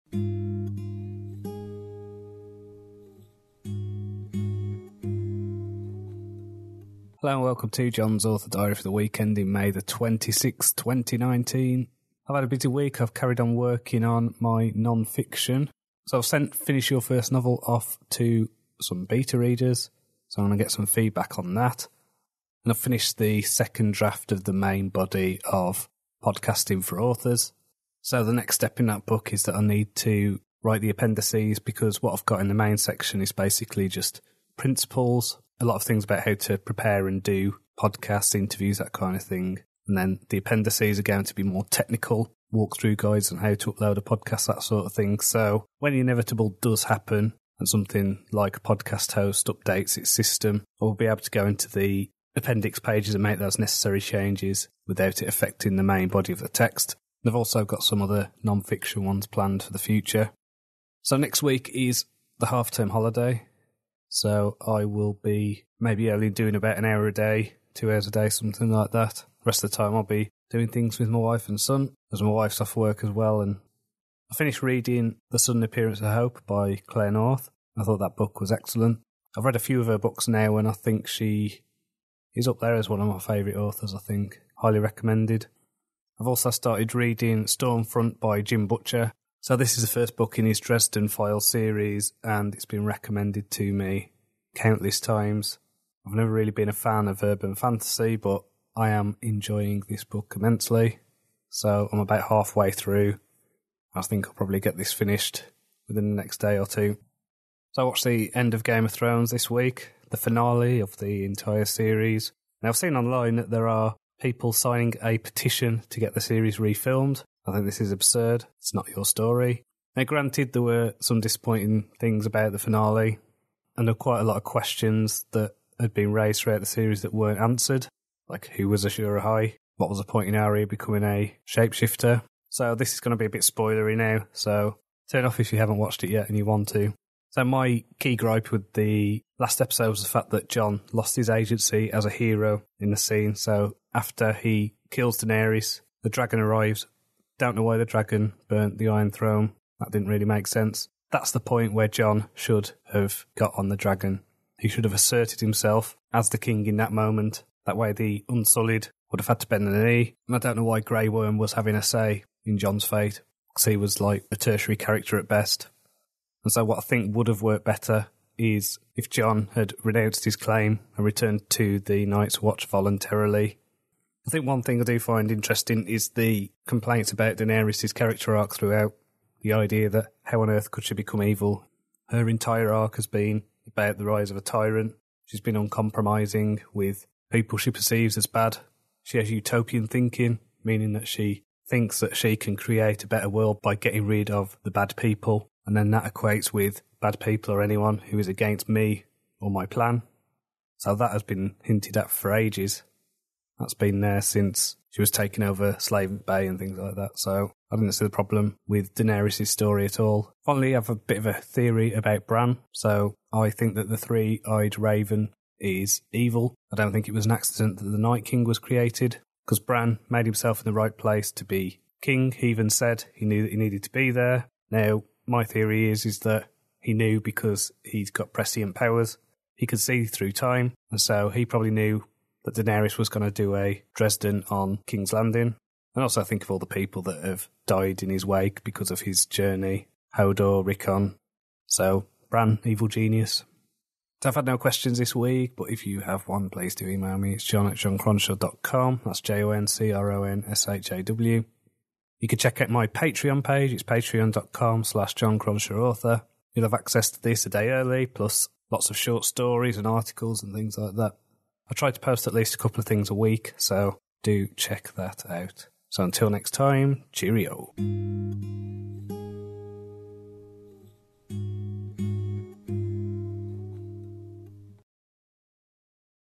Jon's Author Diary - 086 [ May 26, 2019
Welcome to Jon's Author Diary for the week ending May 26, 2019.

 I carried on writing my non-fiction books. I sent Finish Your First Novel off to my beta readers to get some feedback and finished the second draft of the main body of Podcasting for Authors.

 The next step in that book is to write the appendices because the main section is principles on preparing a podcast and how to source and conduct interviews, etc. The appendices are going to be more technical walk-through guides on how to record, edit, upload, and promote a podcast. So when inevitable does happen and something like a podcast host updates its system, I’ll be able to go into the appendix pages and make those necessary changes without affecting the main body of the text.

 Next week is the half term holiday, so I will only be doing about an hour a day as I plan on spending time with my wife and son.

 After that, I plan on returning to my Ravenglass Chronicles series to get the second draft of The Hermit finished.

 I’ve also started reading  Storm Front by Jim Butcher. This is the first book in his Dresden Files series and it's been recommended to me countless times over the year. I’ve never been a fan of urban fantasy, but I am enjoying this book immensely.

 I watched the finale of Game of Thrones this past week and I have a theory.

 *HERE BE SPOILERS*

 I’m convinced that Bran is a deeply evil character—if it is even Bran at all who inhabits his body. Because of his powers and the fact that he knew where he had to be and why, I’m convinced he must have known what would happen to King’s Landing.

 He could have prevented a million deaths and he didn’t.

 He was and still is the three-eyed raven. And the creation of the Night King was no accident.

 There is no other way around it—Bran is evil.